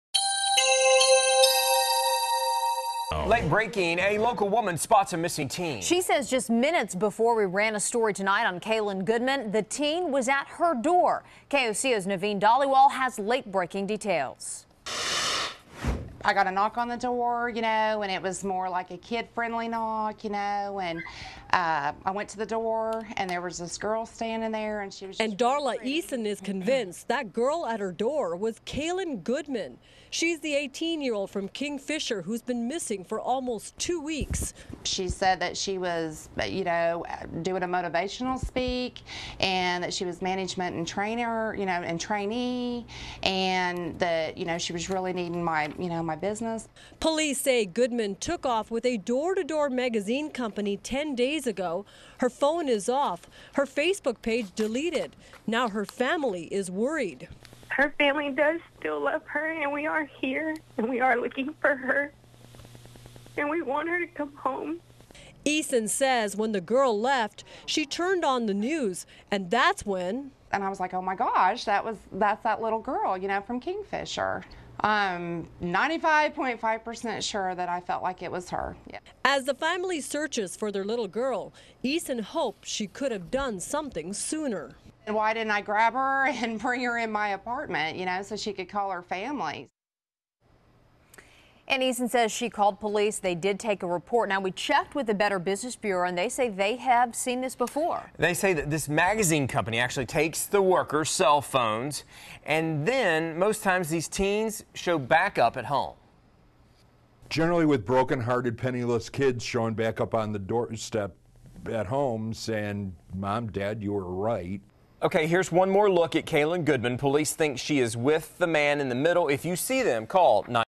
Local Woman Says She Saw Missing Teen
A local woman says she saw a missing Kingfisher teen.  Naveen Dhaliwal reports.